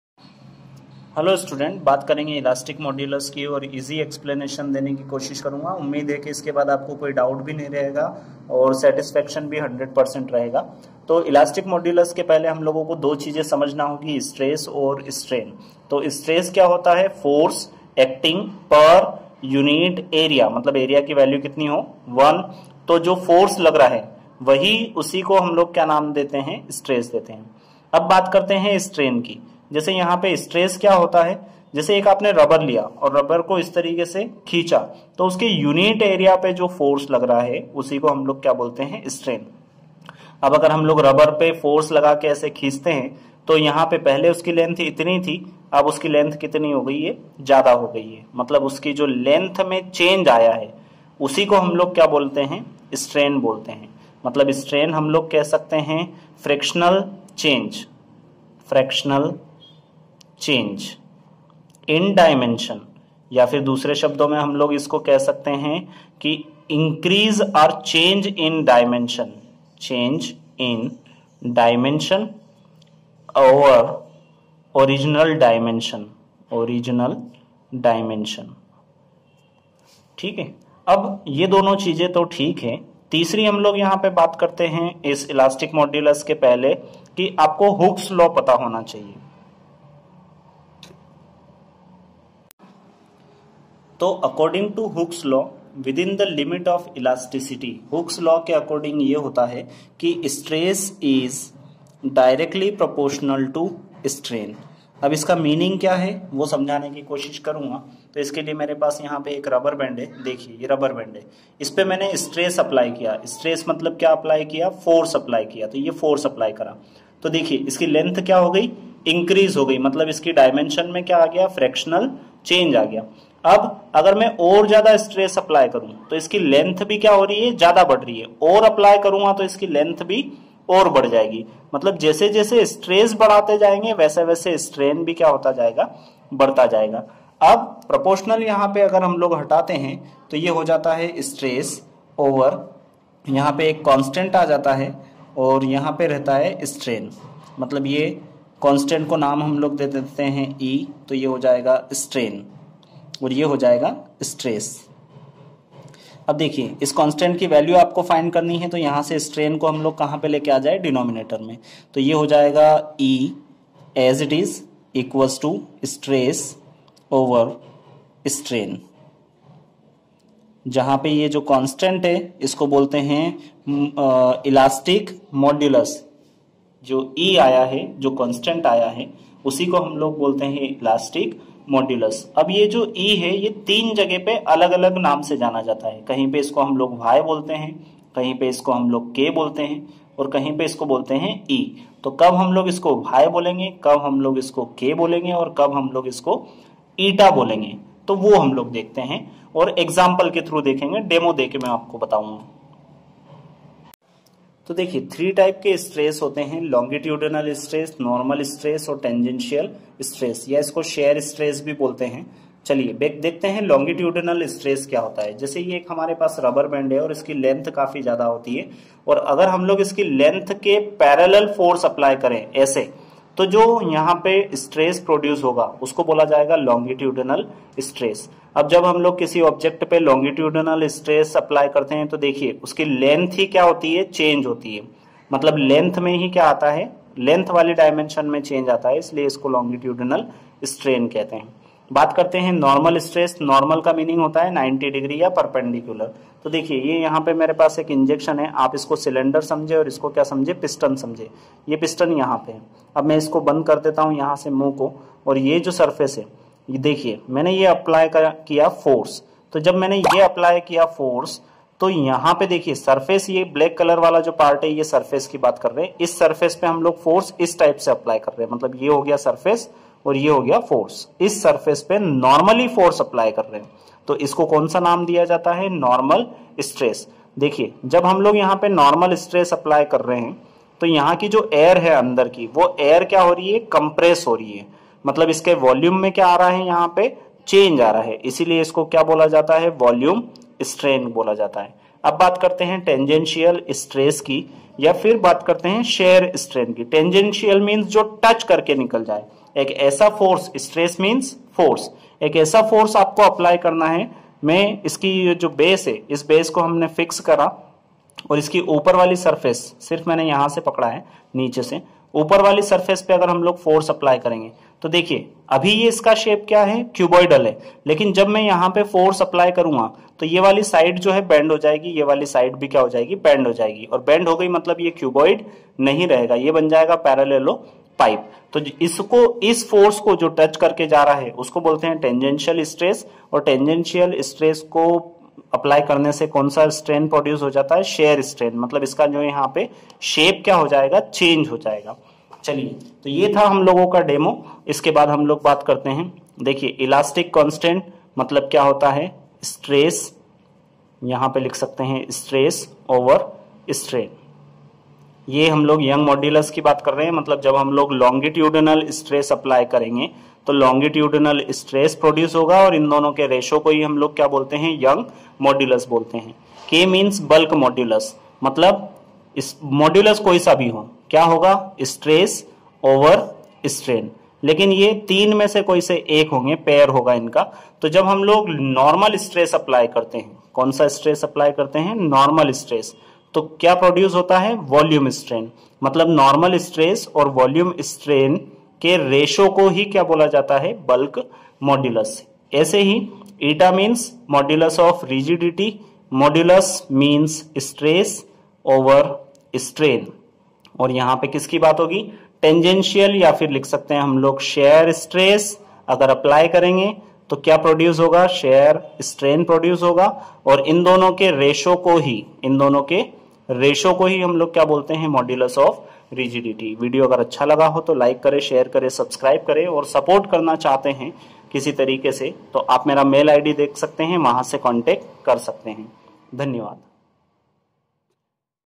what is Hooks law and Elastic constant of a matter in hindi//physics||class11
video deals
1. Stress and strain
2. Hooks Law
3. elastic constant.
4. Youngs modulus
5. Bulk modulus
6. Modulus of Rigidity
also demonstrate the longitudinal stress and corresponding strain,
normal stress and volume strain
tangential(shear) stress and shear strain.